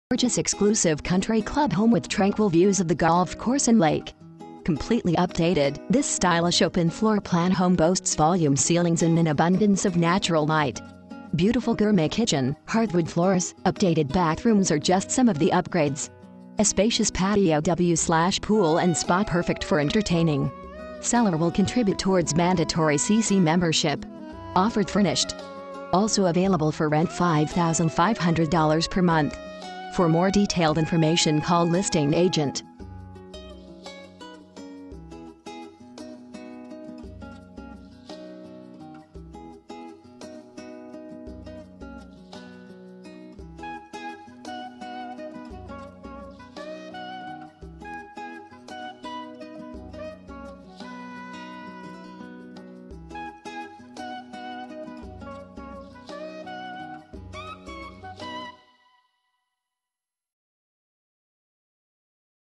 10714 Kirkaldy Ln Boca Raton FL
GORGEOUS EXCLUSIVE COUNTRY CLUB HOME WITH TRANQUIL VIEWS OF THE GOLF COURSE & LAKE  COMPLETELY UPDATED, THIS STYLISH OPEN FLOOR PLAN HOME BOASTS VOLUME CEILINGS AND AN ABUNDANCE OF NATURAL LIGHT  BEAUTIFUL GOURMET KITCHEN, HARDWOOD FLOORS, UPDATED BATHROOMS ARE JUST SOME OF THE UPGRADES  A SPACIOUS PATIO W/POOL & SPA PERFECT FOR ENTERTAINING  SELLER WILL CONTRIBUTE TOWARDS MANDATORY CC MEMBERSHIP  OFFERED FURNISHED ALSO AVAILABLE FOR RENT $5,50000 PER MONTH FOR MORE DETAILED INFORMATION CALL LISTING AGENT

Video uploaded on Sun 01 May, 2011
real estate Boca Raton FL